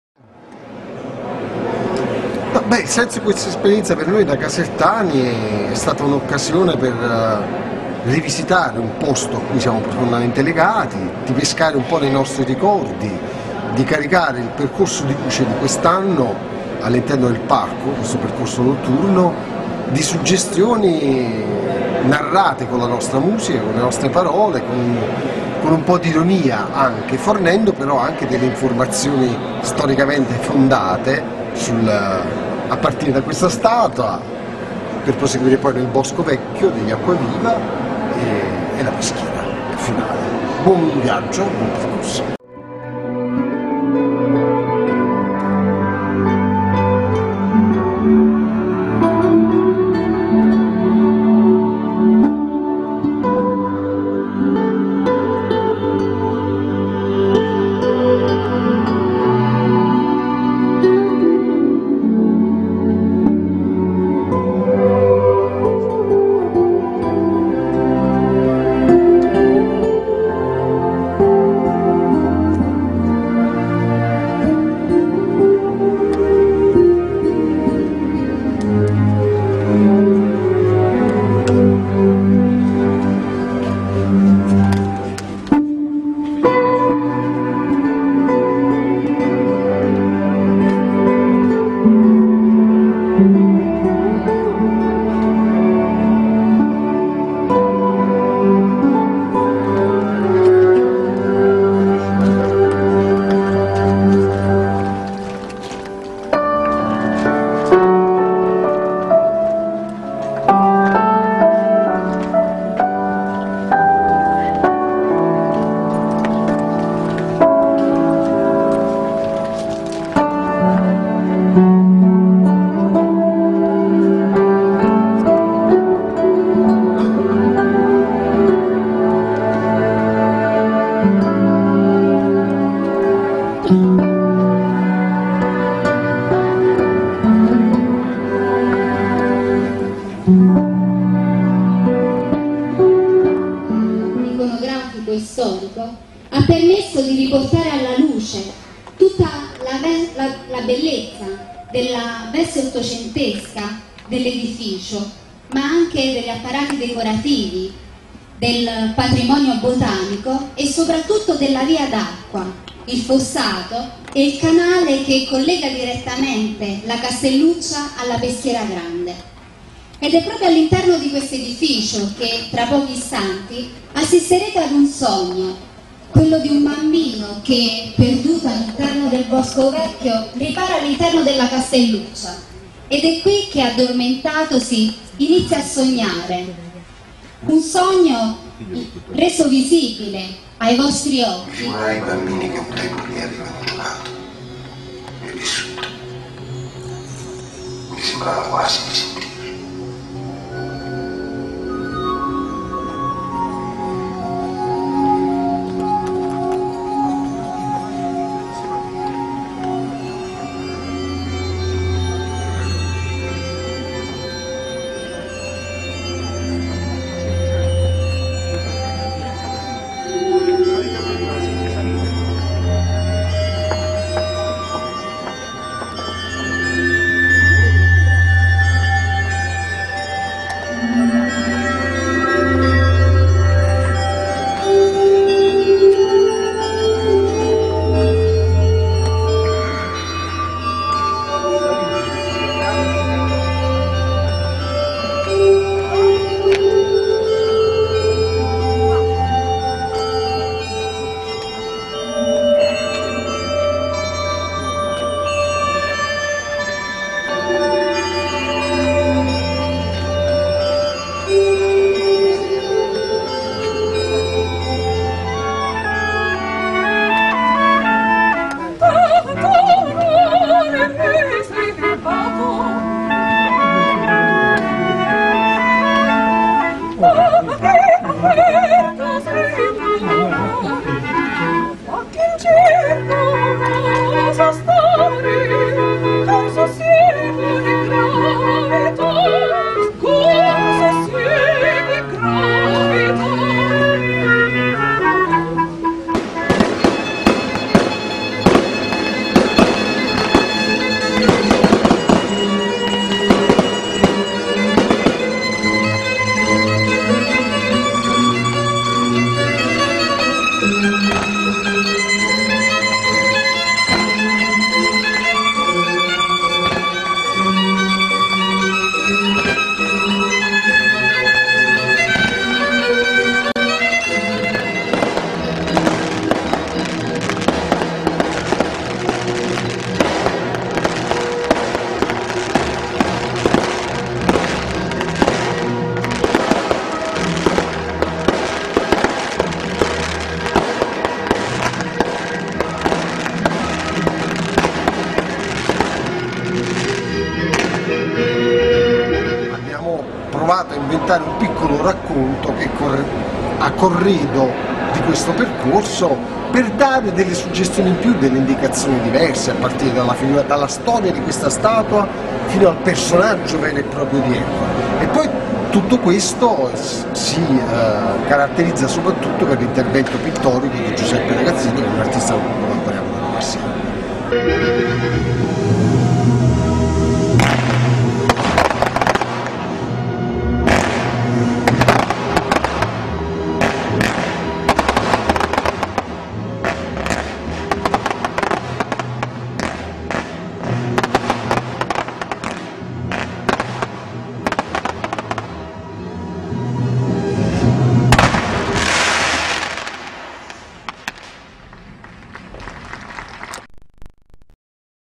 Caserta - Inaugurazione Percorsi di Luce alla Reggia, intervista a Peppe Servillo
- Caserta - Inaugurazione Percorsi di Luce alla Reggia
Intervista a Peppe Servillo, passeggiata nel parco reale e fuochi d'artificio sulla peschiera grande.
Prima della conferenza stampa di presentazione nel Teatri di Corte. (25.06.10)